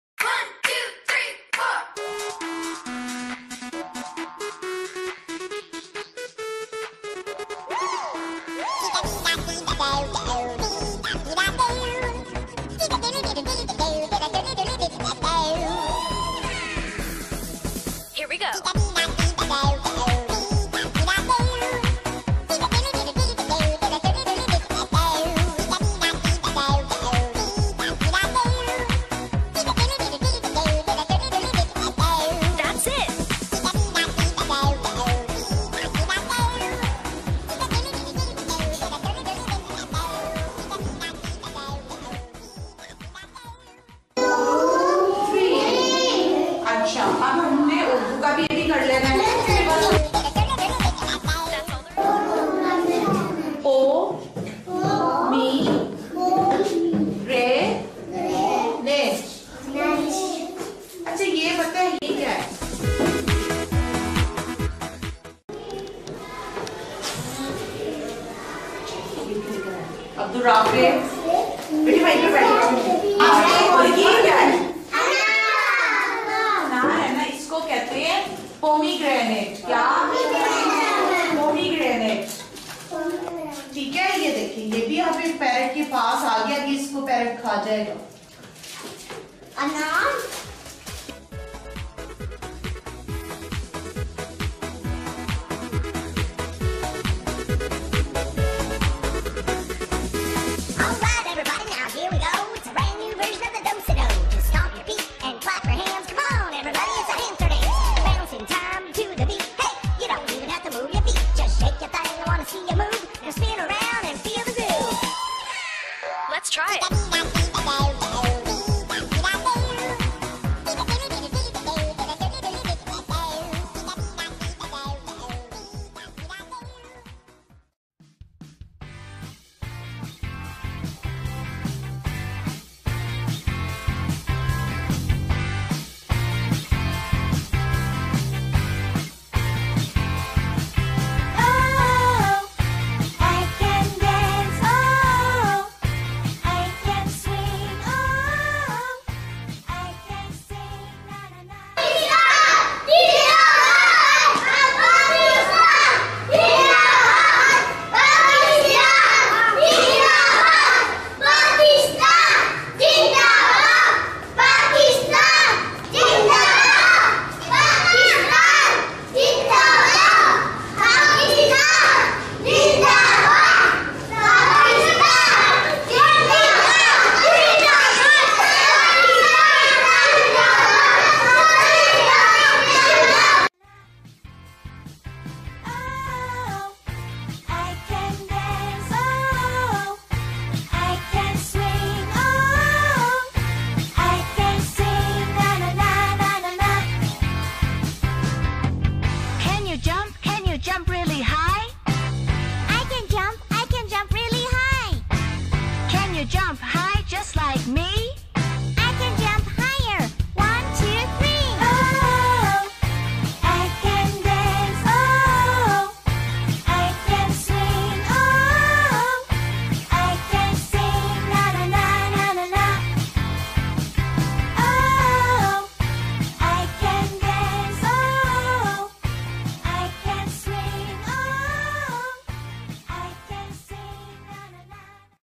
Early Childhood Learning at Gems Education
In this video, Abdul Rafy is doing work and enjoying with friends
LEARNING WITH FUN AT GEMS EDUCATION.